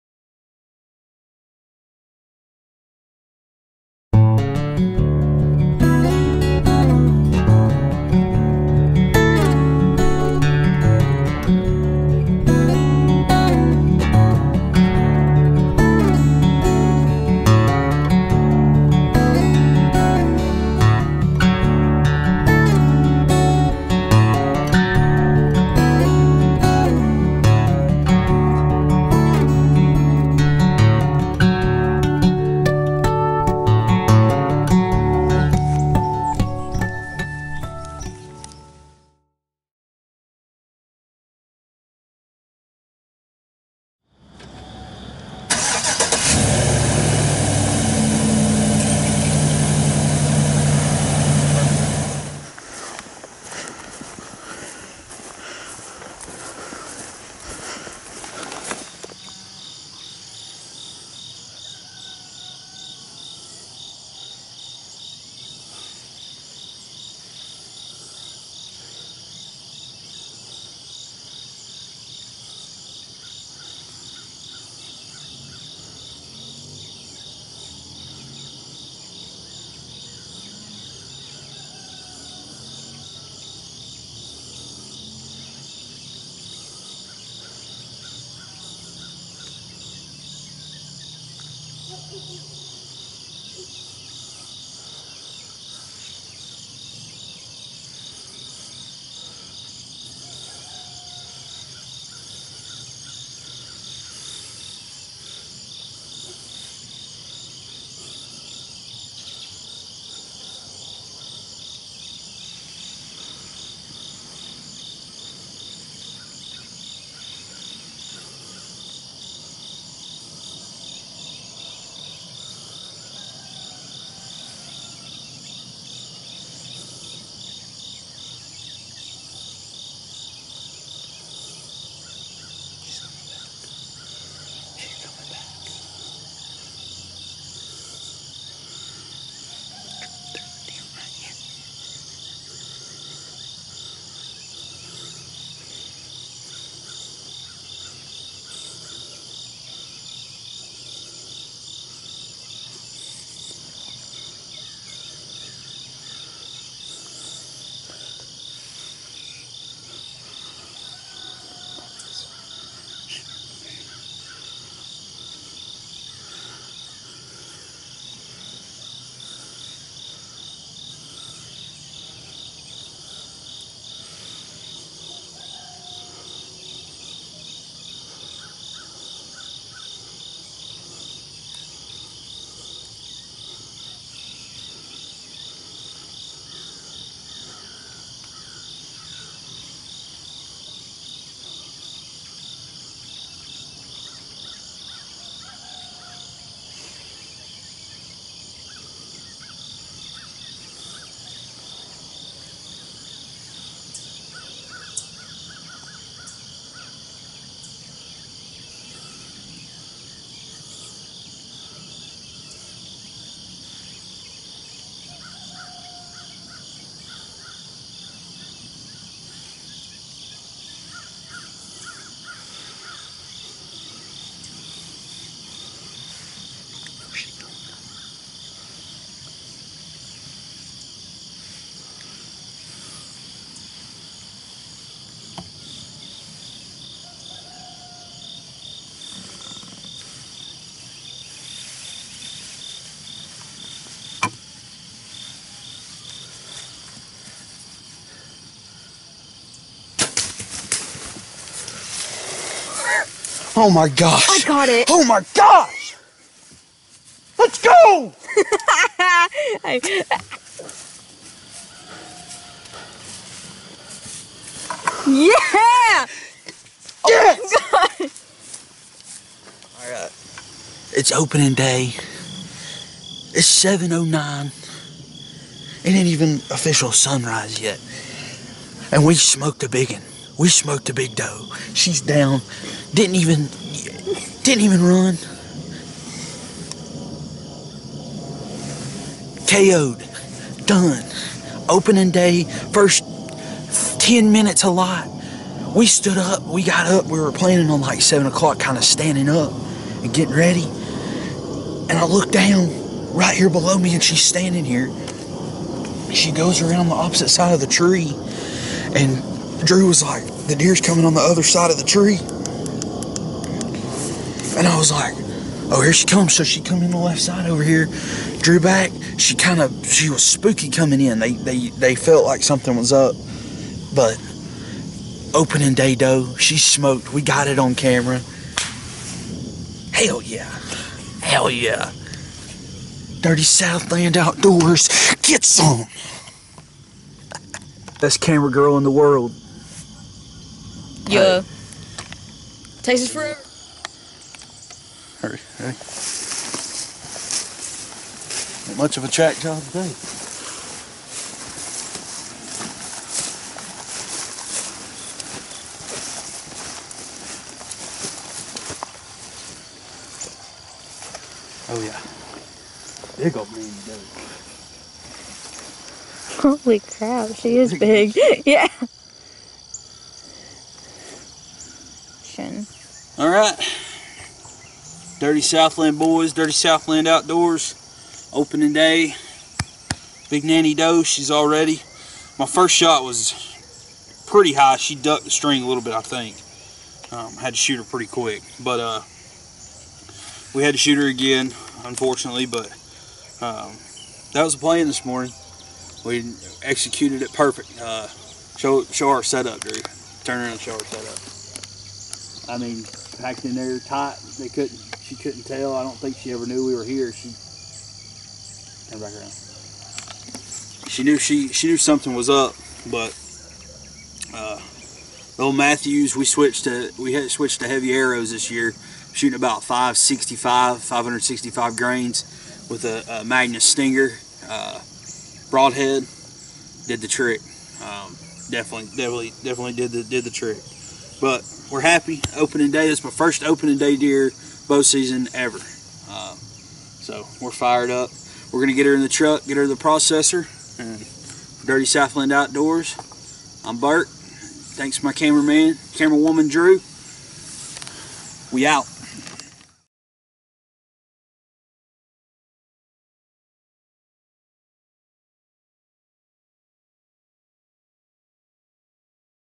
Opening Day Doe in Georgia!!
Dirty Southland Outdoors first video. Kicking it off with Prostaffer Chase Burke getting it done on opening day in Georgia.